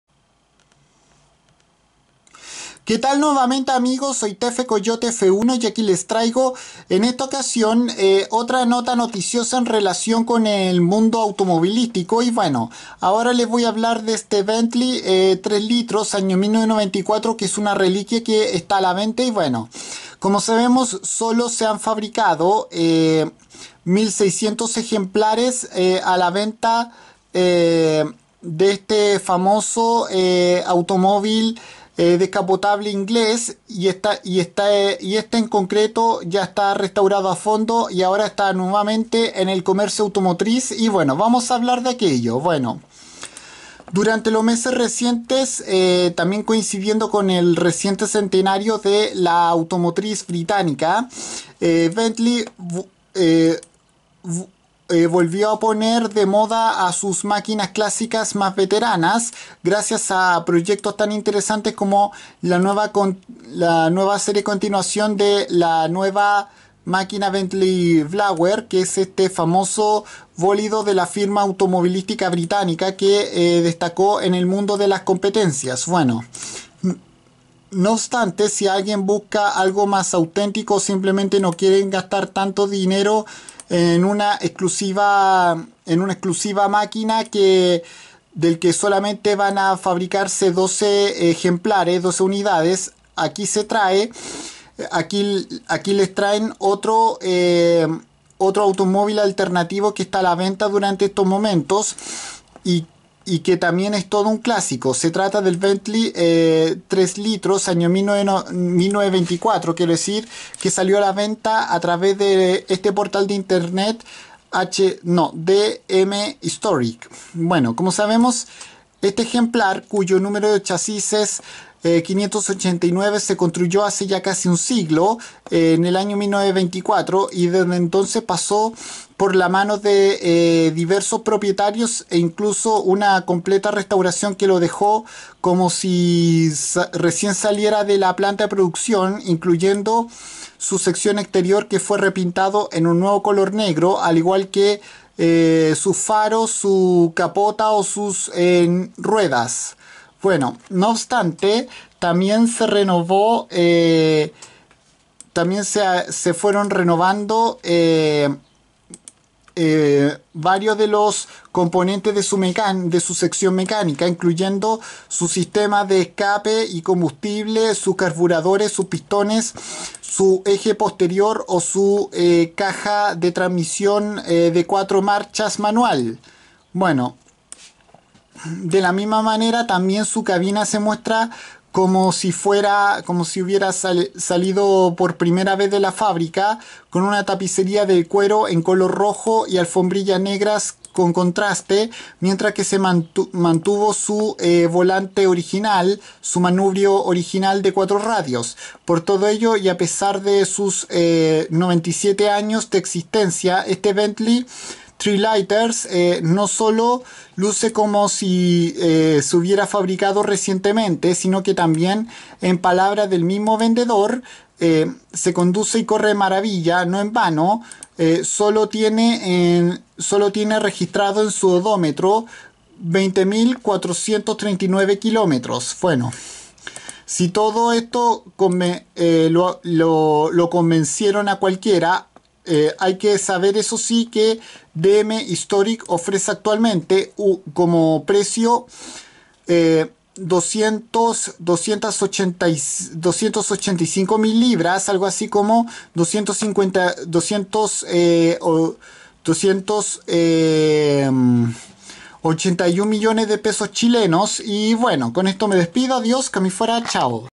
En venta este Bentley 3 Litros de 1924
En este vídeo les comento acerca de este automóvil descapotable inglés del cual solo hubieron 1.600 ejemplares y que luce un aspecto muy impecable ya que fue recientemente restaurado